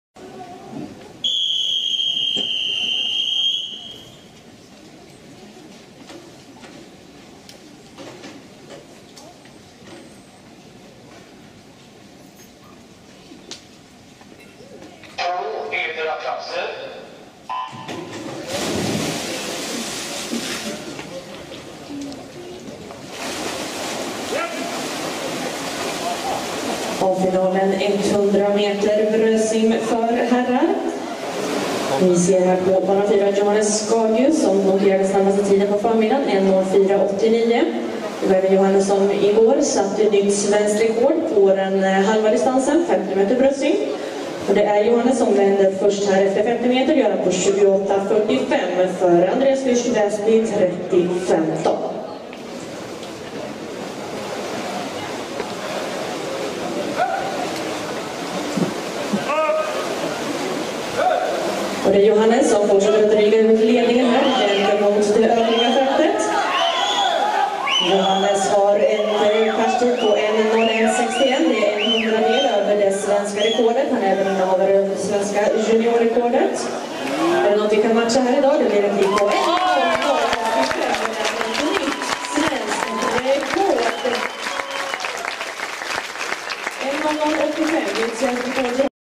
NEW SWEDISH RECORD Men's 100 Breaststroke Swedish Grand Prix 2013
18-year old Johannes Skagius really began emerging into a true star by breaking the Swedish National Records in both the 50 and the 100 breaststrokes- 1:00.85